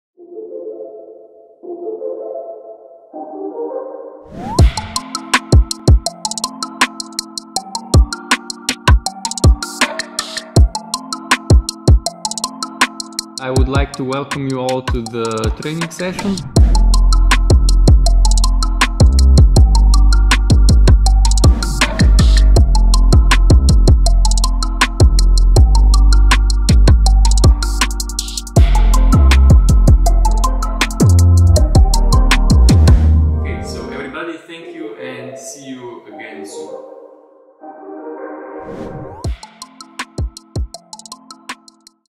Cultural Heritage Protection Training - Short Promo
Between 15 and 17 September, the Centre for European Perspective conducted an online pilot training on the Protection of Cultural Heritage in EU missions and operations.

The training was designed for a wide range of participants who will be deployed to EU international missions and operations in third countries in the near future. Based on coordination with European partners and a preliminary needs analysis prepared by the Center for the Protection of the Cultural Heritage of Italian Carabinieri, the Center for European Perspective designed a new training course and recently conducted a pilot training.